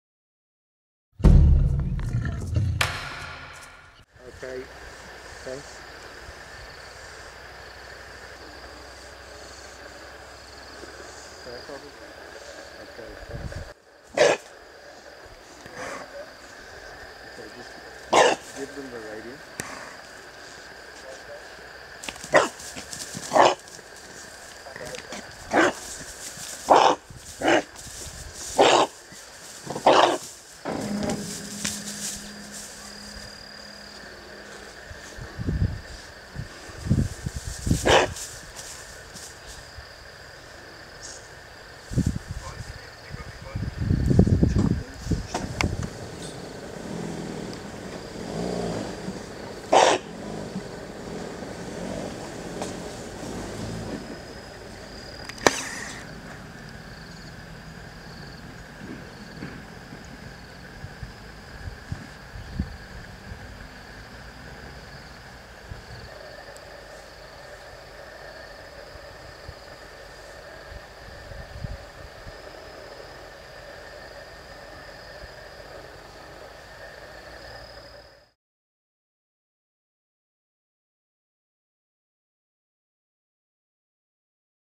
Python Strangles Young Bushbuck Whilst Mother Tries to Free It - Londolozi TV
A large African Rock Python strangles its captured prey (a young bushbuck) in the long grass, whilst the bushbucks mother desperately tries to free its offpspring by barking and biting at the pythons body.  Filmed by: Rich Laburn at Londolozi Game Reserve, bordering the Kruger National Park in South Africa.